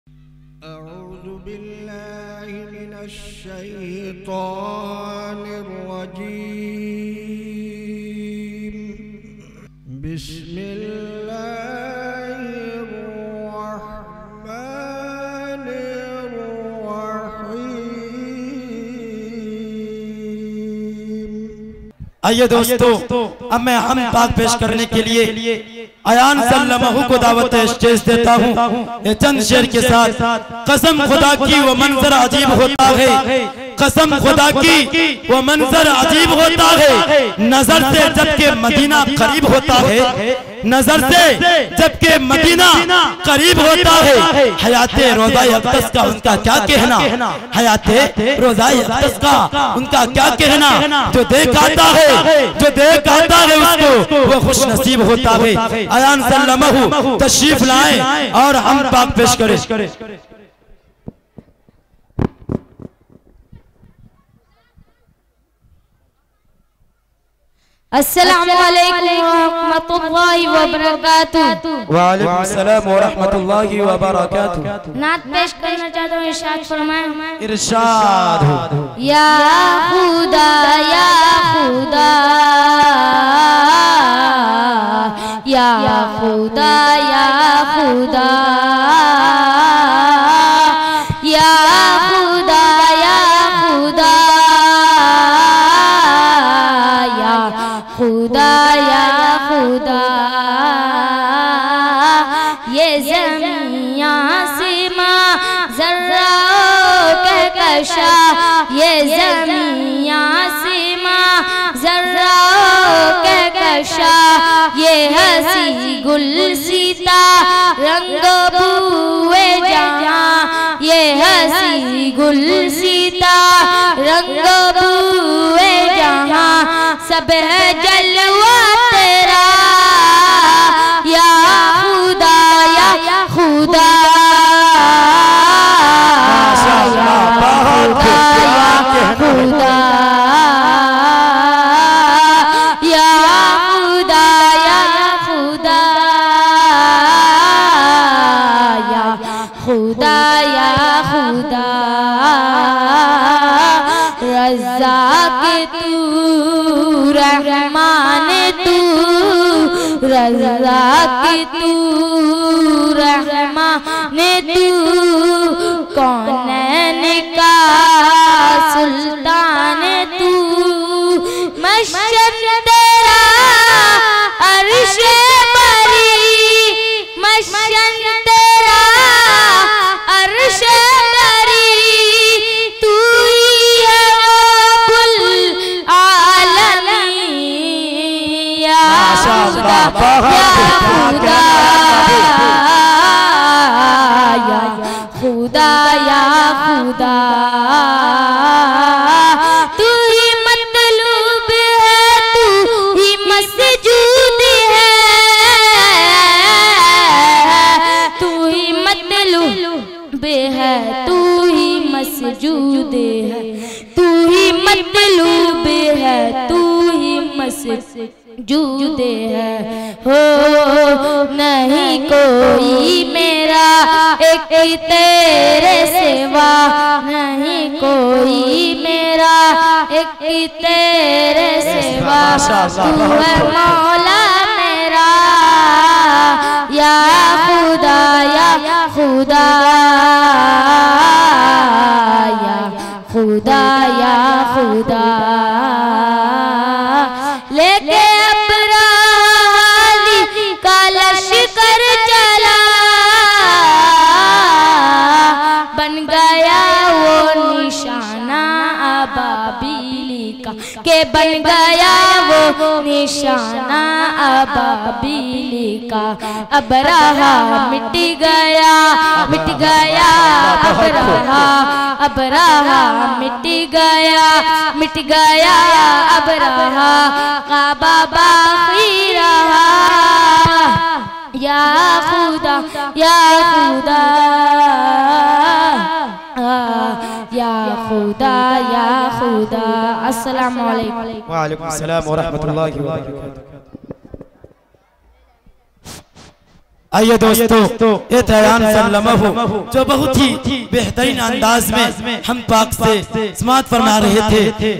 Mohammad Ayan Jalsa Dastar Bandi Madrasa Islamia Arabia Kako Jahanabad Bihar 18-02-2024
Mohammad Ayan
Jalsa Dastar Bandi Madrasa Islamia Arabia Kako Jahanabad Bihar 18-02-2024
Nazim e Jalsa: Altaf Zia Gayavi
Nazim e Madrasa: Md Haidar Ali
Sadar e Madrasa: Mumtazul Haque
Khajanchi: Sayyed Sultan Sb
Secretary: Jamal Mazhari
Ustaz e Madrasa: Md Ashraf Ali
Hafiz Wasiullah Sb
Qari Asrar Sb
Venue: Kako Jahanabad Bihar
Videography & Uploaded By JK Live Media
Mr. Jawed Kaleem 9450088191